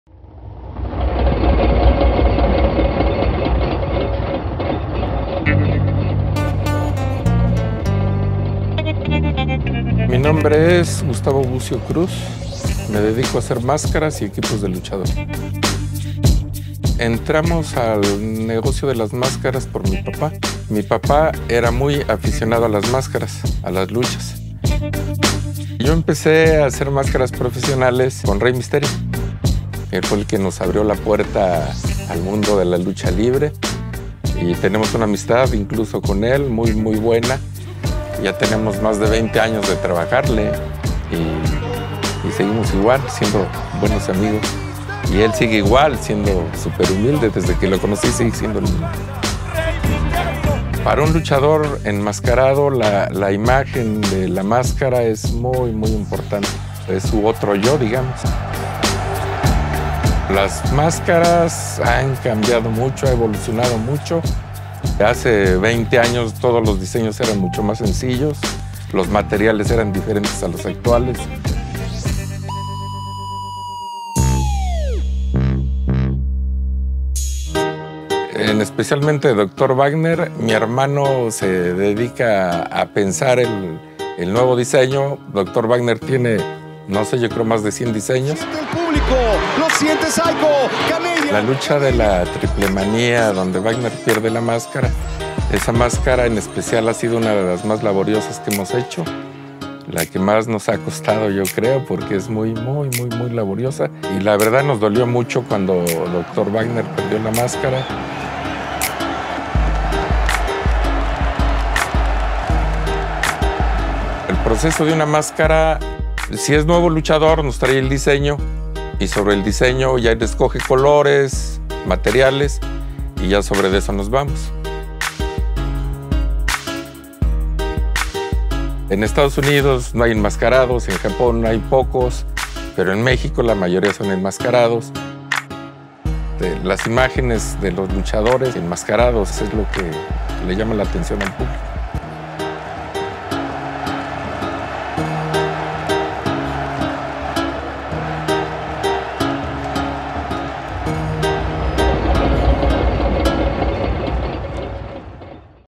Entrevista con el Mascarero de Lucha Libre AAA "Gustavo Boscio"
Conoce el proceso de realización de las mascaras de lucha libre mexicana. Descubre como se hicieron las máscaras famosos de Rey Misterio, así como de Doctor Wagner que perdió en Triplemania